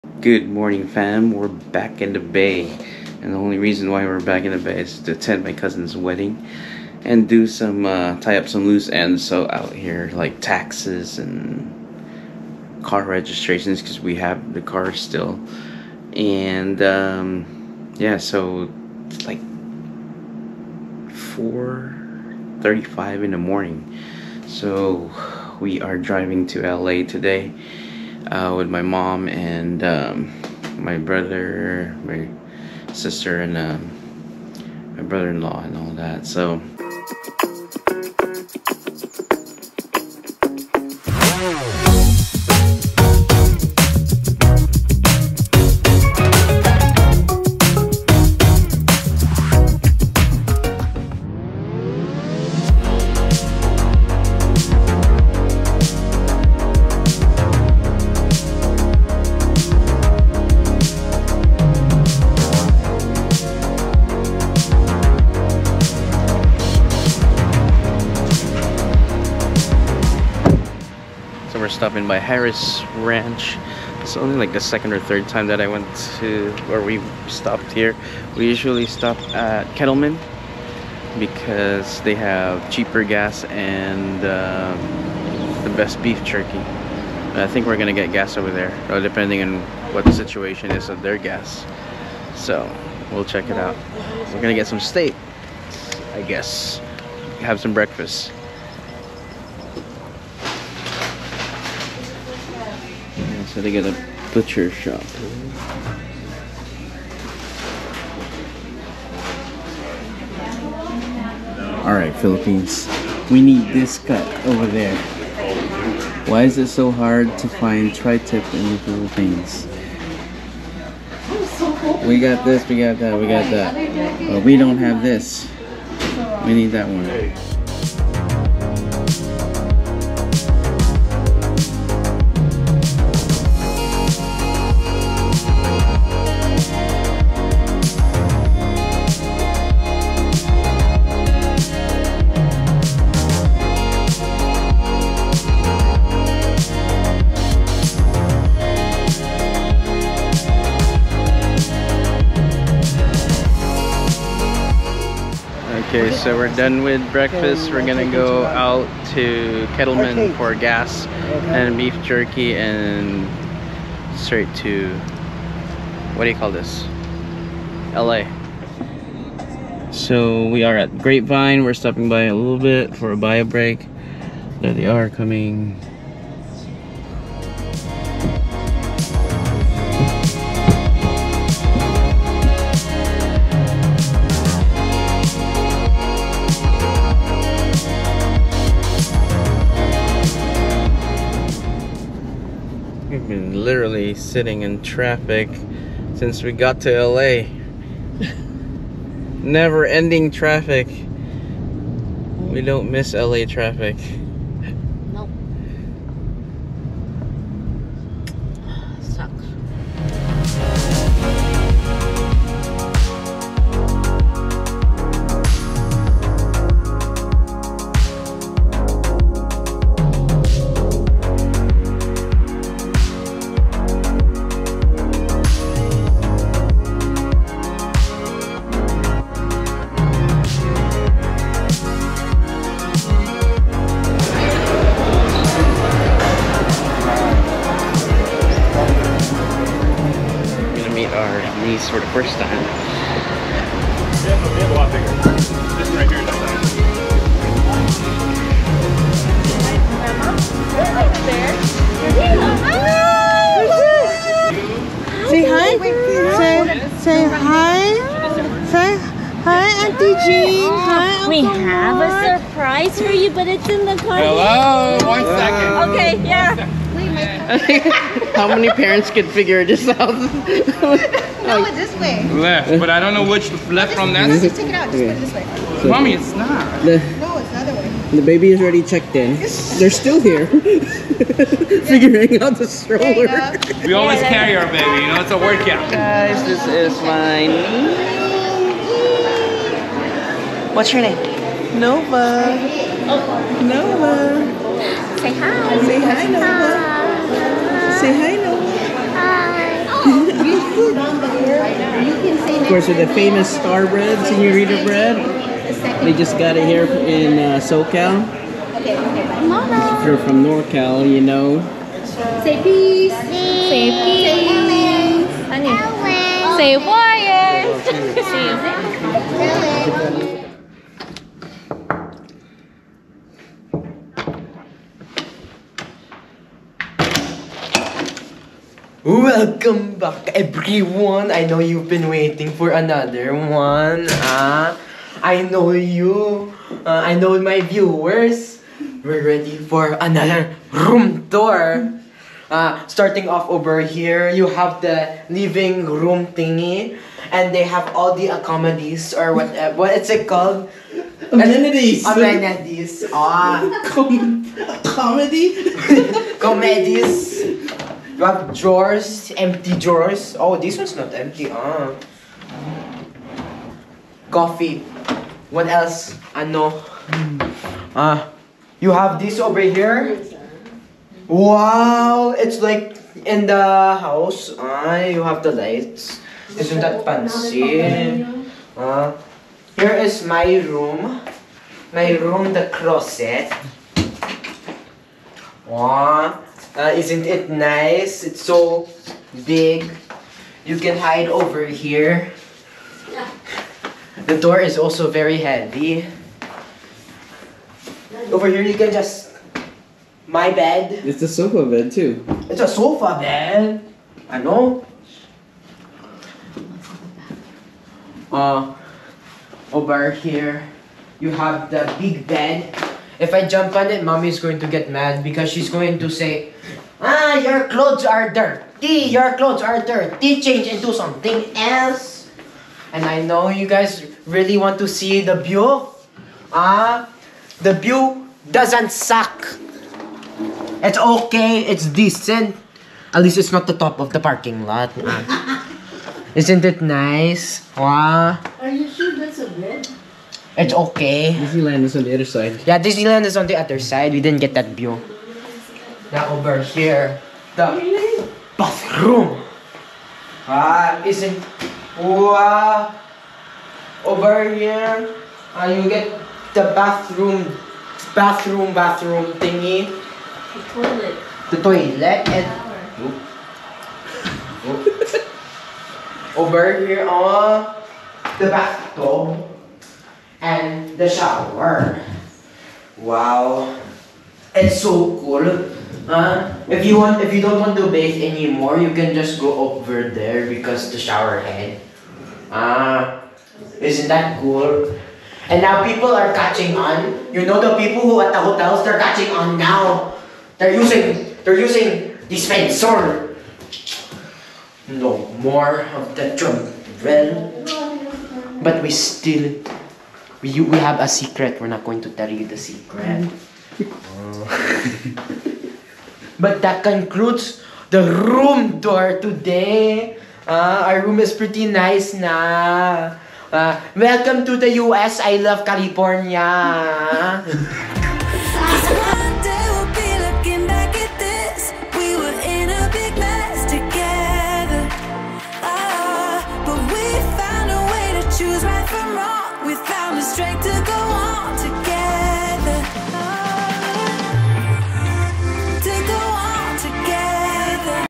Roadtrip to LA | Fam Vlog #118
Time to head down to Southern California for our cousin's wedding. However, this LA traffic probably just got worst since we last drove down. Seeing the family and especially first time seeing our niece was well worth it!